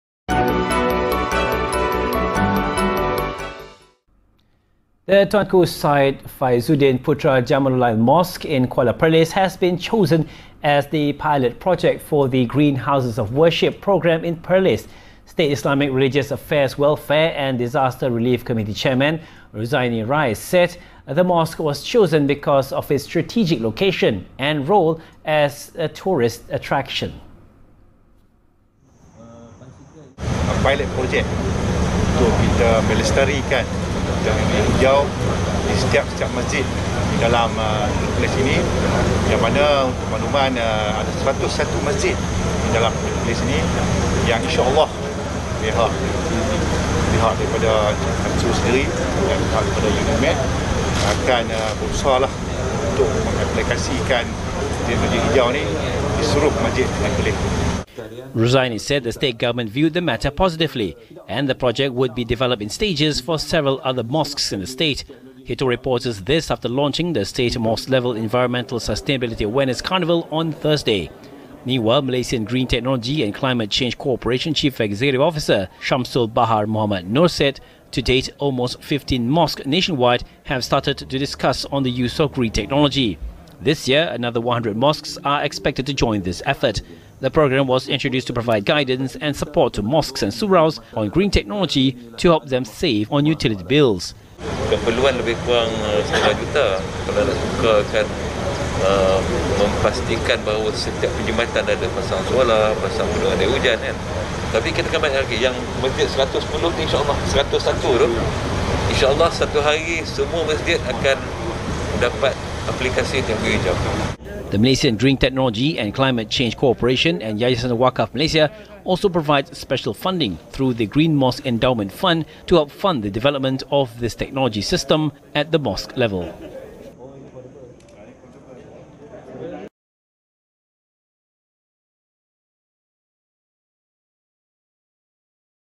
Tuanku Syed Faizuddin Putra Jamalullail Mosque chosen for pilot project of Green Houses of Worship
BernamaNews: The Tuanku Syed Faizuddin Putra Jamalullail Mosque in Kuala Perlis has been chosen as the pilot project for the Green Houses of Worship programme in Perlis.

State Islamic Religious Affairs, Welfare and Disaster Relief Committee chairman Ruzaini Rais said the mosque was chosen because of its strategic location and role as a tourist attraction.